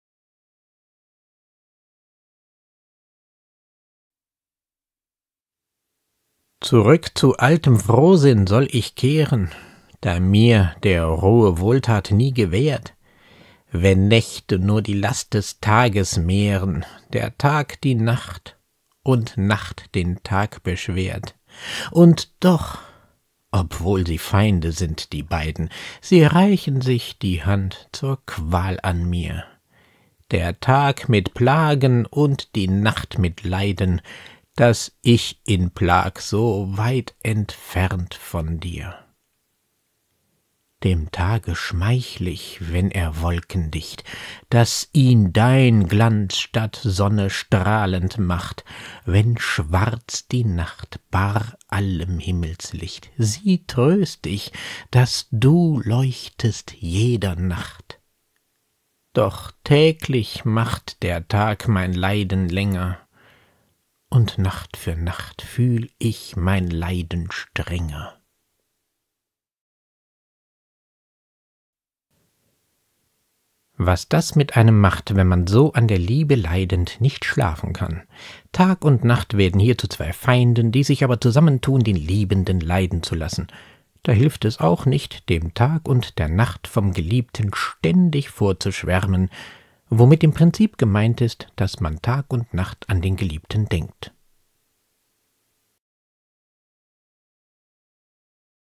Shakespeare: Sonett 28 (deutsch) - How can I then return in happy plight (in eigener Nachdichtung)
Was das mit einem macht, wenn man so an der Liebe leidend nicht schlafen kann. Tag und Nacht werden hier zu zwei Feinden, die sich aber zusammentun, den Liebenden leiden zu lassen. Da hilft es auch nicht, dem Tag und der Nacht vom Geliebten ständig vorzuschwärmen, womit im Prinzip gemeint ist, dass man Tag und Nacht an den Geliebten denkt.